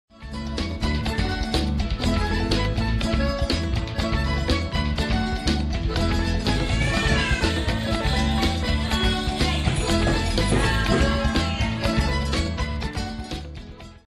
BV Toddlers roll playing and Dancing
Who would you like to be today? Our toddlers got the chance to dress up as popular animals and dance around the classroom to drums.